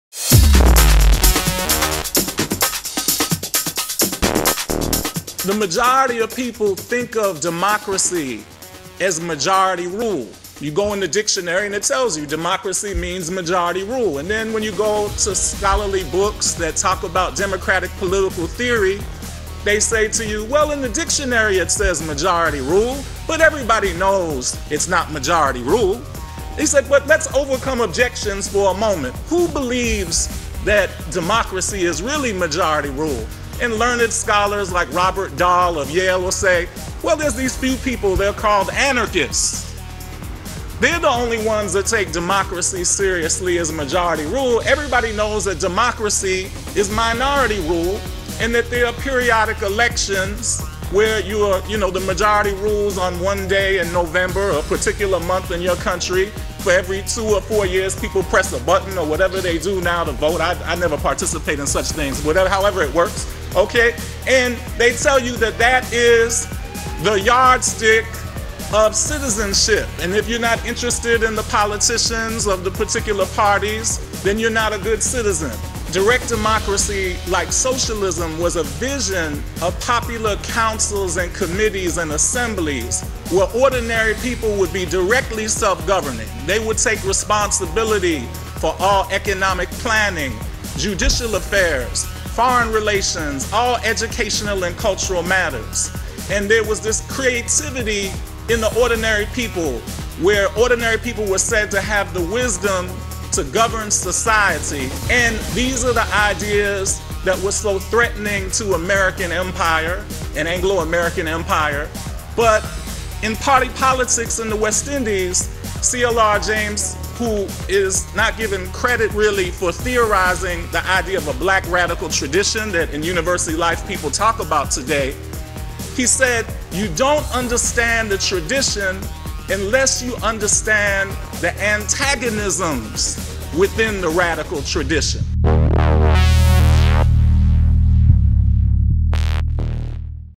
Elevate The Debate
ELEVATE THE DEBATE: Is a new HaMatv series featuring excerpts from lectures given here in Antigua & Barbuda.  Matthew Quest,  received his doctorate in American Studies at Brown University, his masters in African Studies at University of Illinois at Urbana, and his bachelors degree in political science and history at Hunter College, City University of New York. He presently is a lecturer of history and Africana Studies at University of Arkansas at Little Rock.  Here is an excerpt from his presentation held at UWI Open Campus in Antigua. It was a part of the  Lecture on Tim Hector: Race, Activism and Politics.